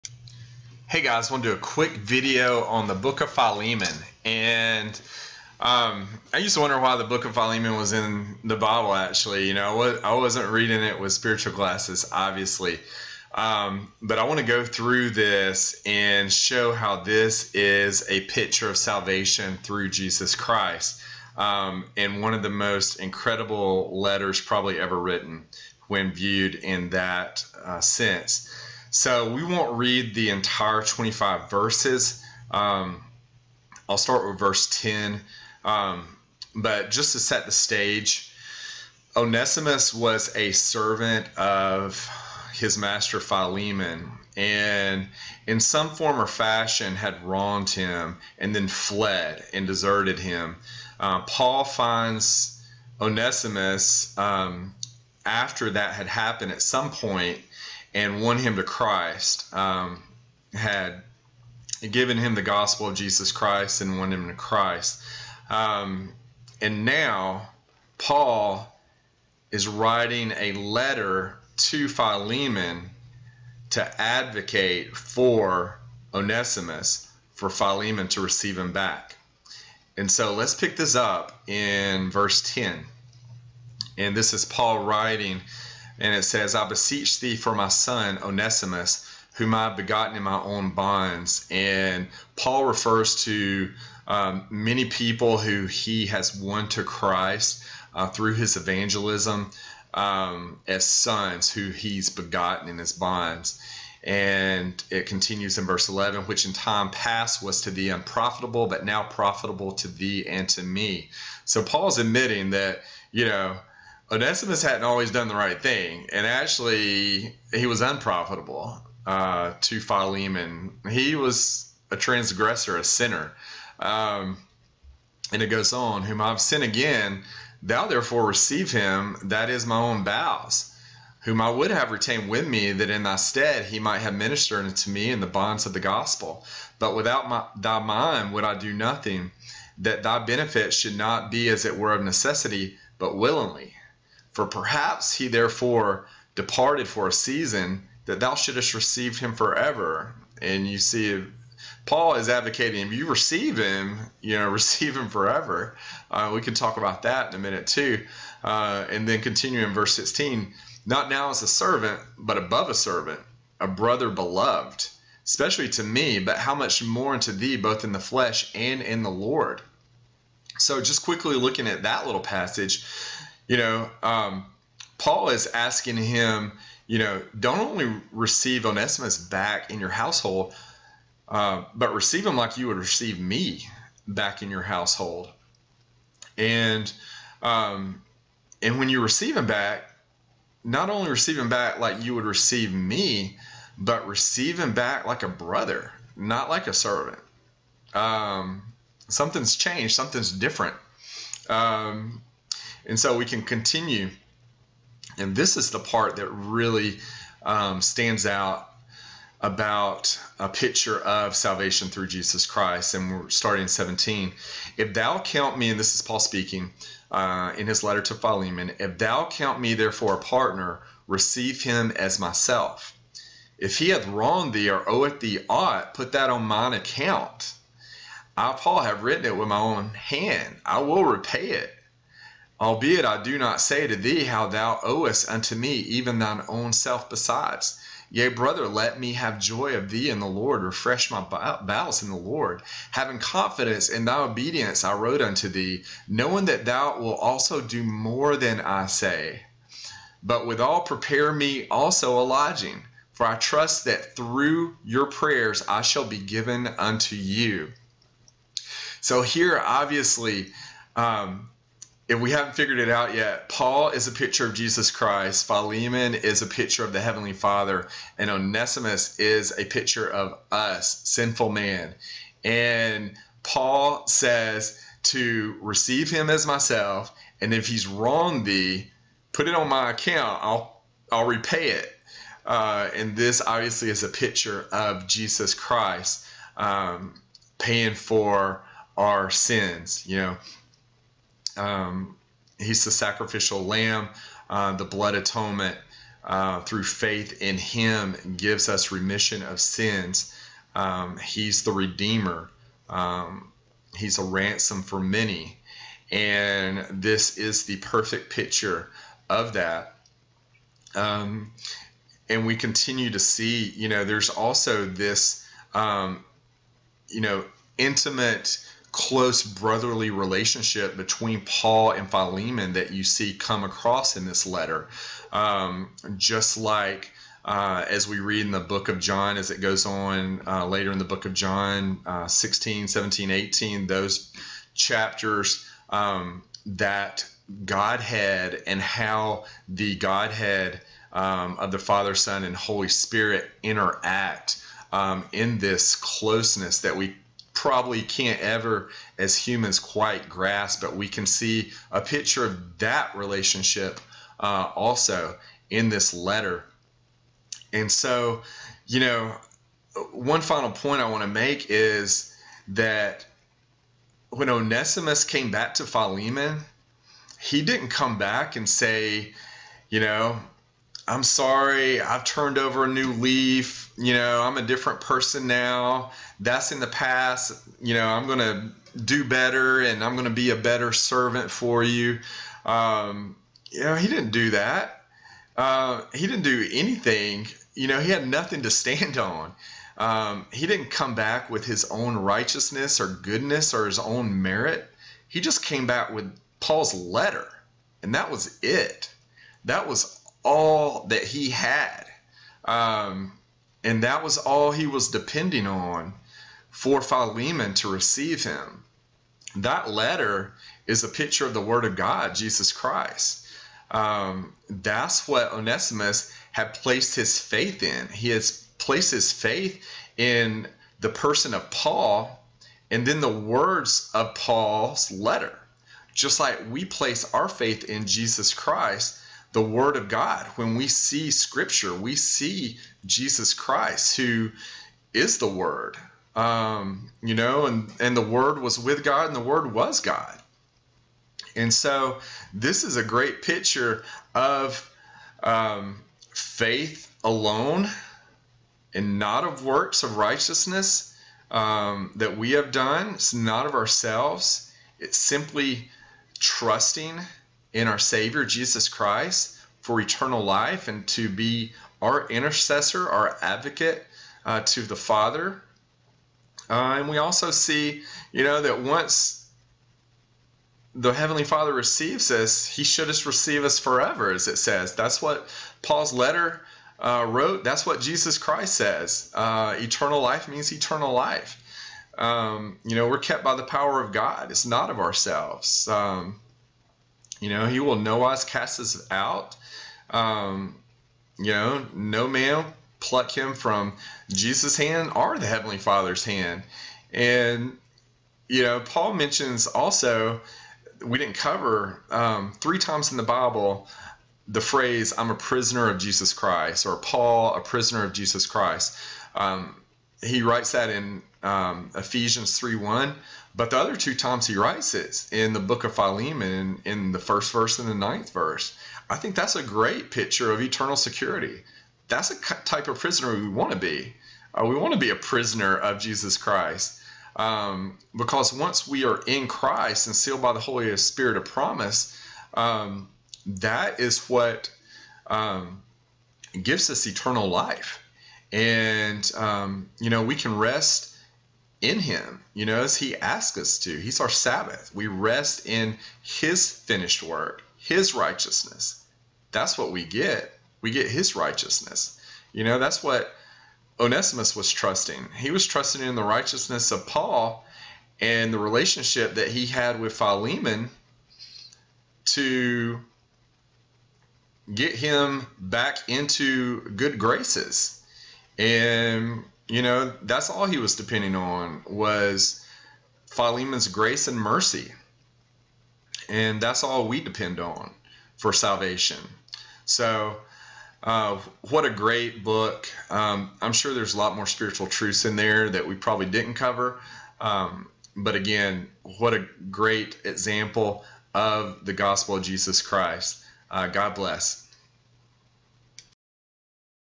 Book of Philemon:A picture of salvation through Jesus Christ
In this video, we look at the Book of Philemon through spiritual glasses.  When used, we see a picture of the Gospel of Jesus Christ jump of the pages of this letter.  Philemon is a type of the Heavenly Father,  Paul is a type of Jesus Christ, and Onesimus is us, sinners.  In this short letter, we see Jesus as our Advocate, our Intercessor, and our Savior.